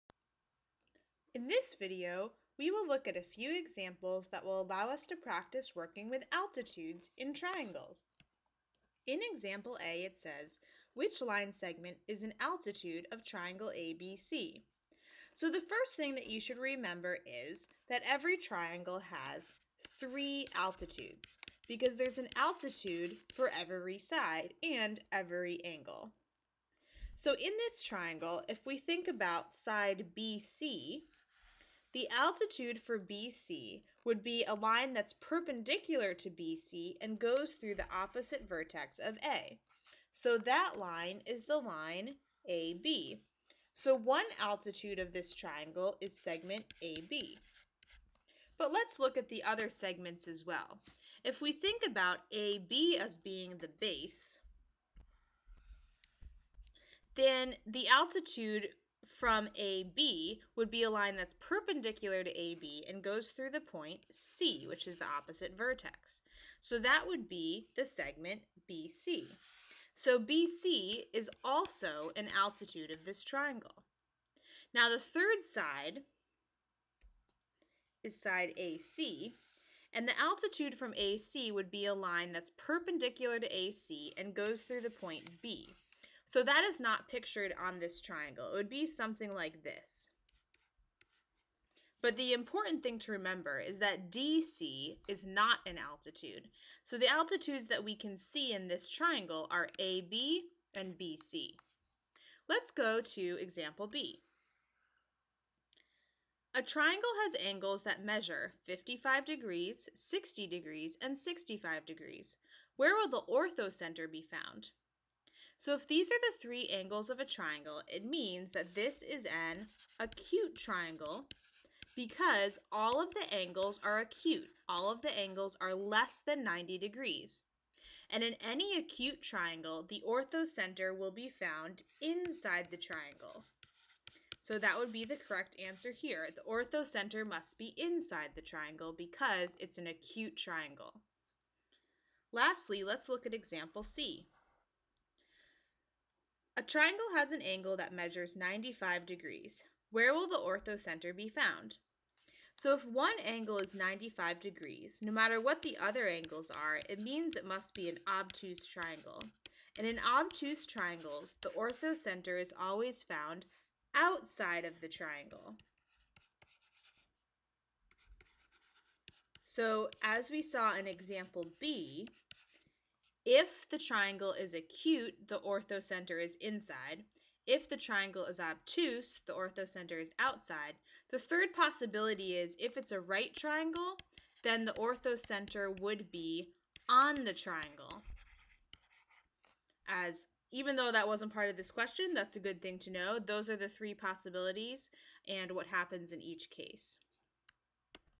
Altitudes: Examples (Geometry Concepts)
Here you'll learn the definition of altitude and how to determine where a triangle's altitude will be found.

This video shows how to work step-by-step through one or more of the examples in Altitudes.